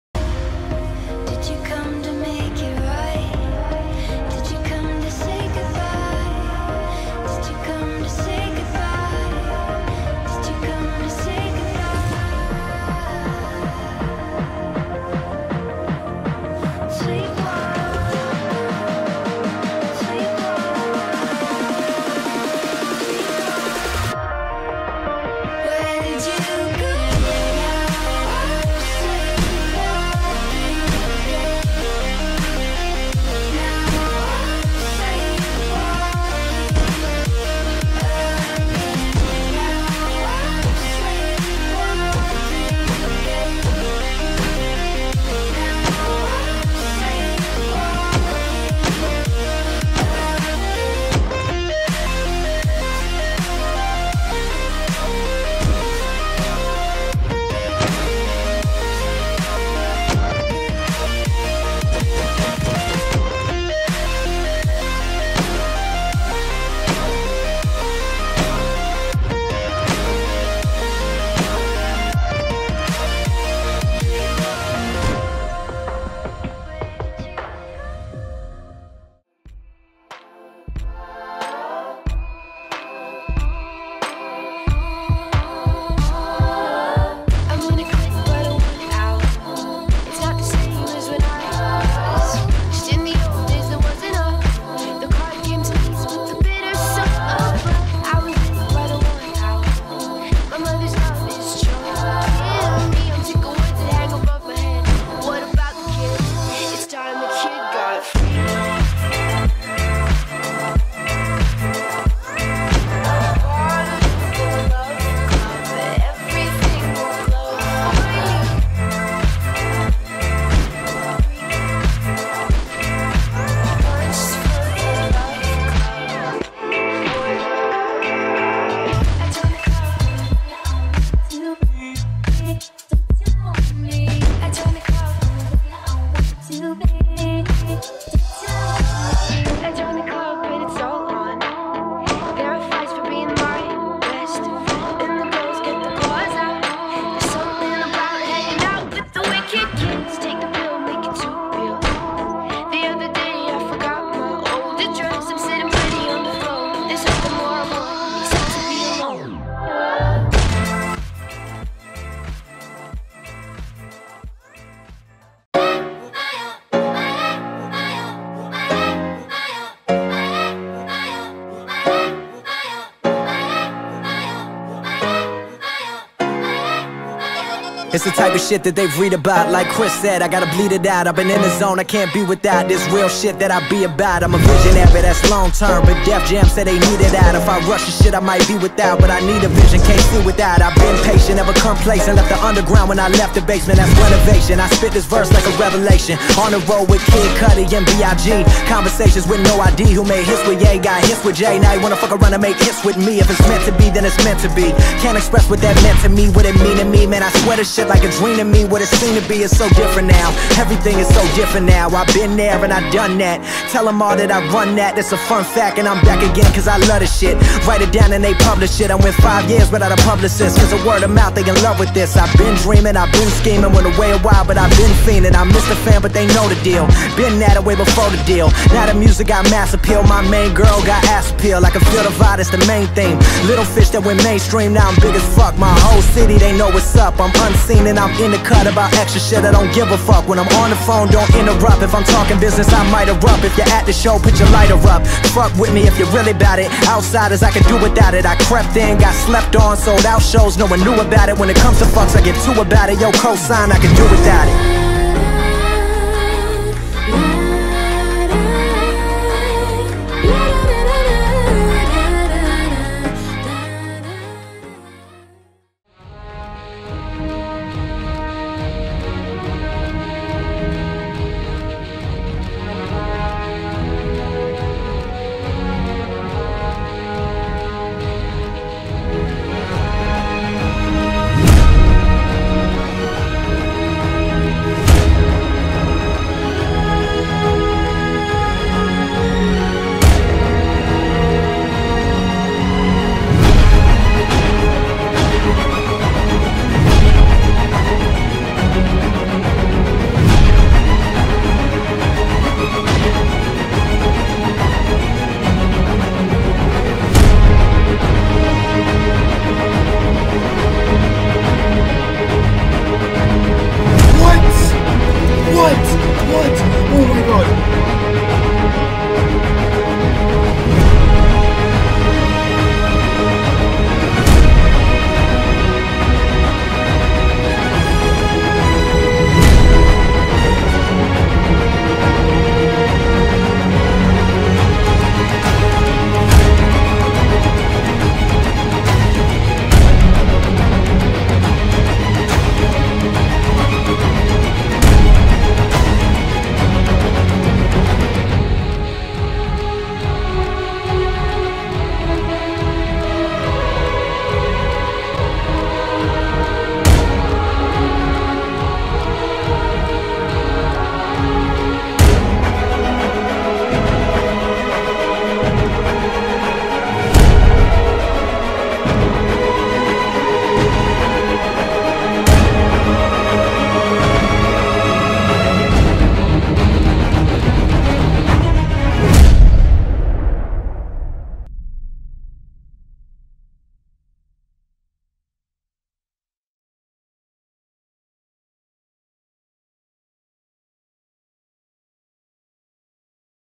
IllUMINESCENT : 100 SUB TEAMTAGE by Fumy XE and Nooby XE
This is by far the biggest project that Xenon Alliance has worked for since we released the recruitment challenge . This team tage took a while to make and all of our members worked extremly hard on this project so please leave a like and subscribe if you enjoyed and prepare for more greatness from XE.